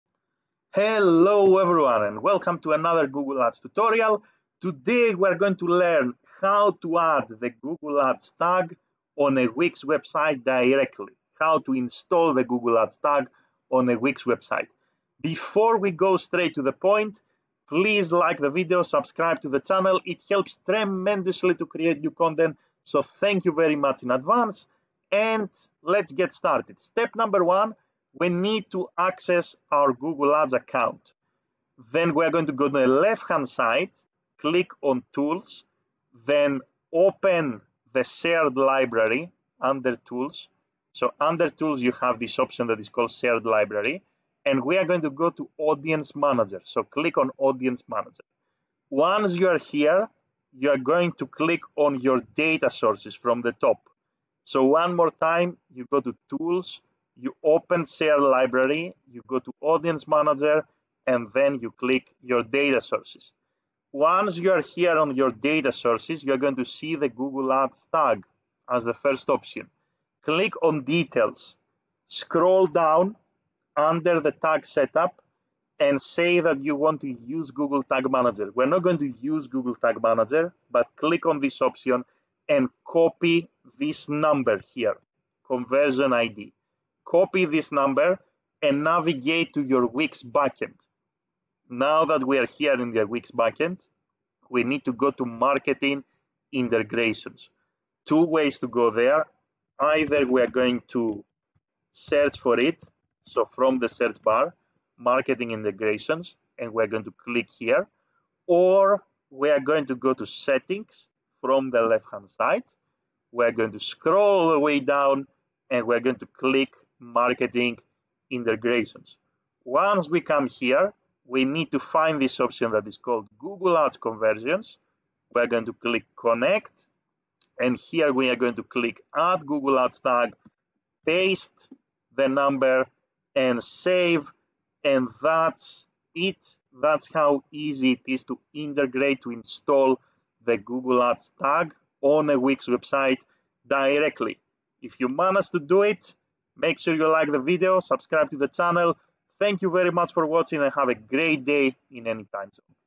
How to Install Google Ads Tag on a WIX Website (Directly)
2) Click on Tools (left-hand-side menu)
3) Open shared library and select Audience Manager
4) Select "Your data sources"
5) Click on Details under your Google Ads tag
6) Scroll down and select Use Google Tag Manager
7) Copy your Conversion ID
8) Navigate to your WIX backend
9) Go to Marketing Integrations
10) Click on "Connect" under Google Ads Conversions
11) Click on "Add Google Ads Tag"
12) Paste the Conversion ID and save